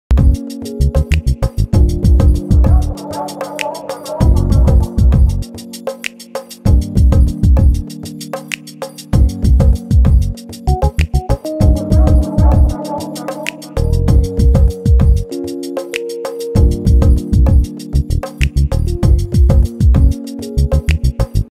PronoviasItBrides: Caroline Bassac - Look 4
Caroline Bassac's fourth look for PronoviasItBrides features the especially flattering Nadira gown with an embellished bodice and full skirt!